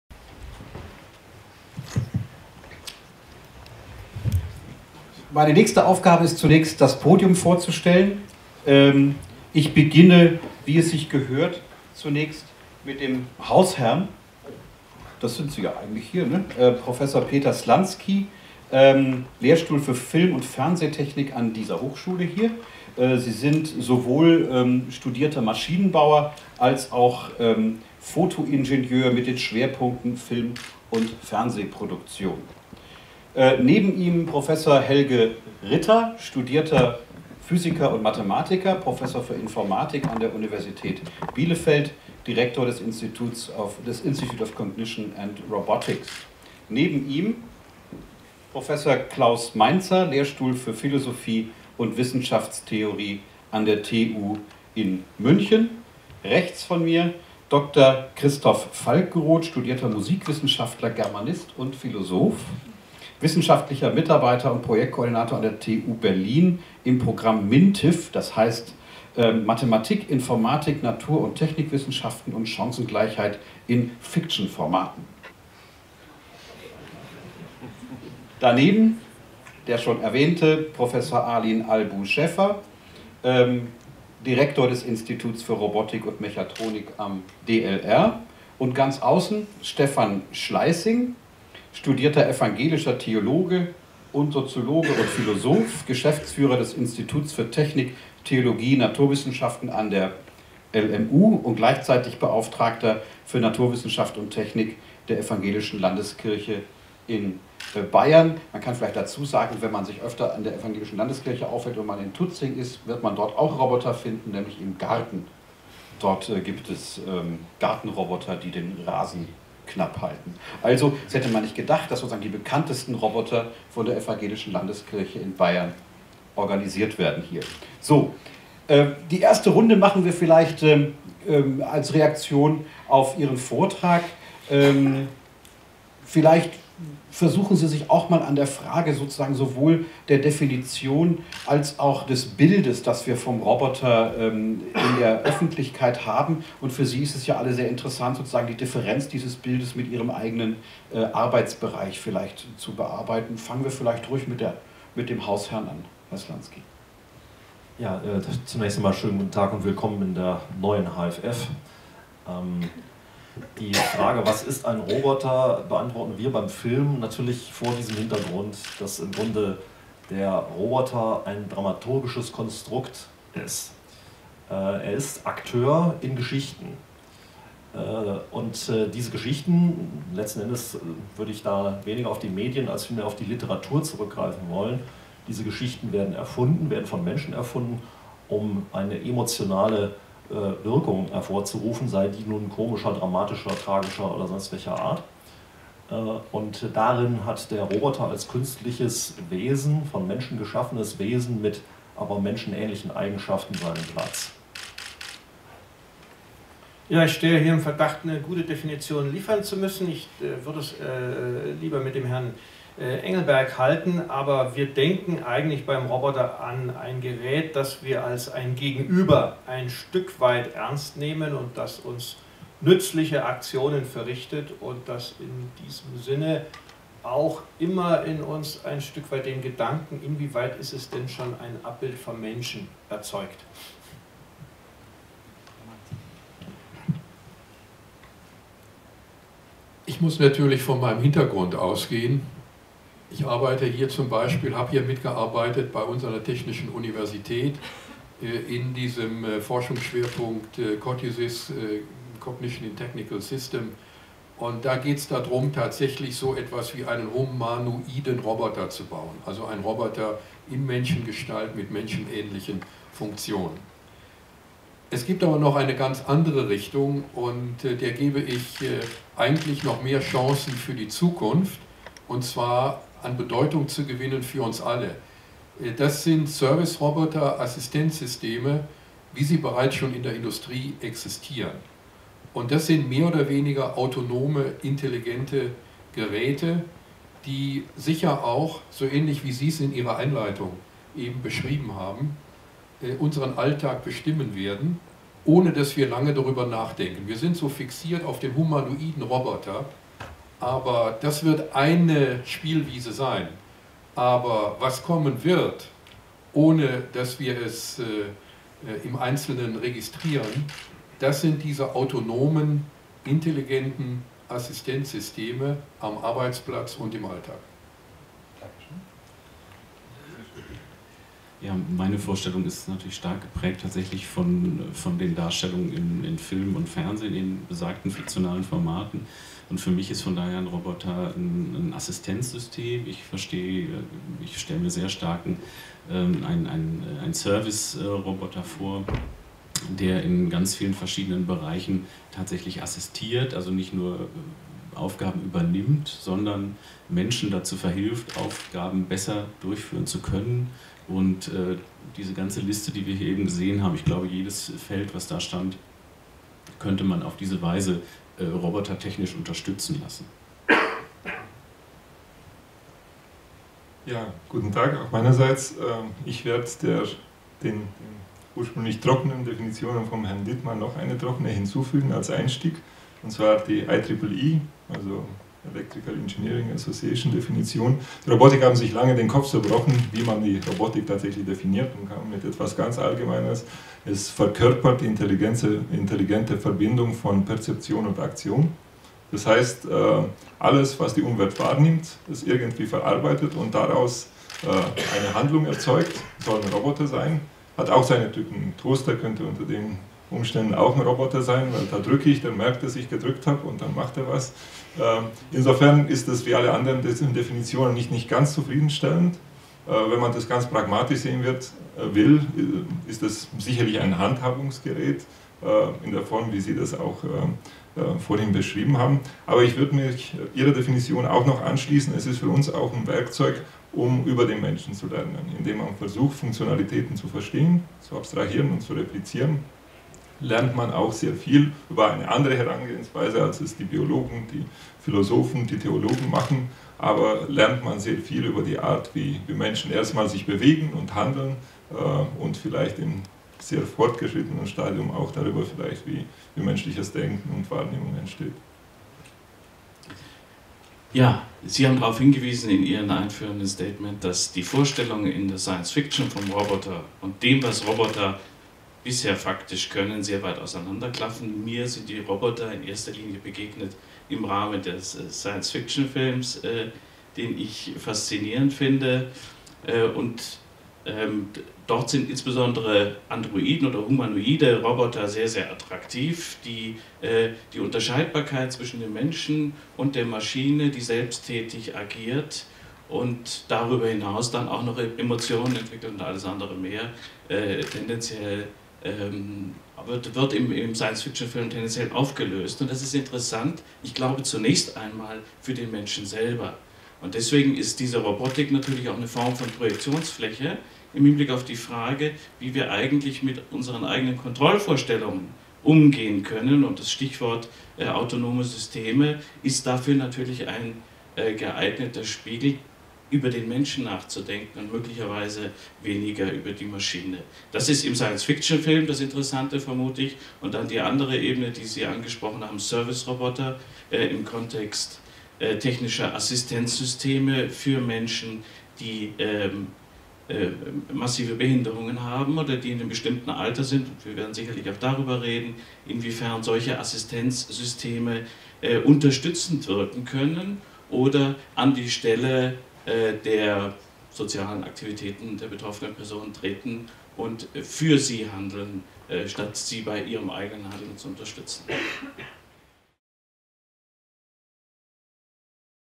Podiumsdiskussion „Roboter: Dein Freund und Helfer?" am 21.04.2013 Teil 2
Teil 2 - Was sind Roboter überhaupt?

Podiumsdiskussion „Roboter: Dein Freund und Helfer?" vom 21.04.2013 in der Hochschule für Film und Fernsehen, veranstaltet vom Institut für Robotik und Mechatronik des Deutschen Zentrums für Luft- und Raumfahrt Oberpfaffenhofen und der Technischen Universität München

Roboter Dein Freund und Helfer?
Wie werden sich Roboter in Zukunft entwickeln? Düstere Gesellen wie sie oft in Science Fiction-Filmen zu sehen sind oder helfende Geister, die unser Leben erleichtern? Experten aus Forschung, Film und Philosophie diskutieren aktuelle Entwicklungen und den heutigen Stand der Forschung gemeinsam auf einer Podiumsdiskussion.